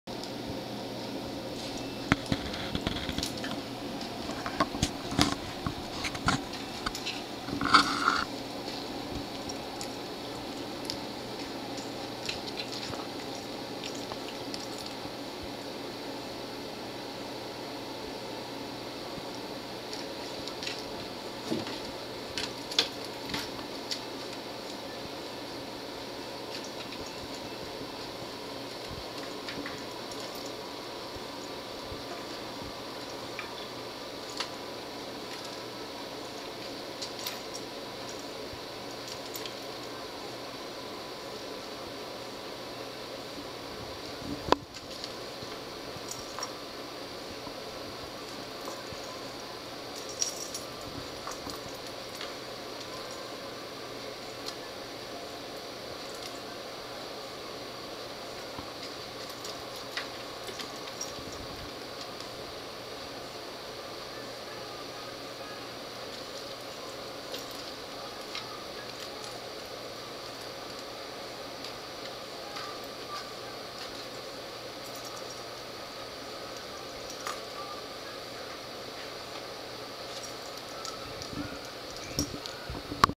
Periwinkle playing with a milk ring
Periwinkle has begun to feel well enough to start pulling out all her toys from her toy box.  Her favorite is the milk ring.  Go figure.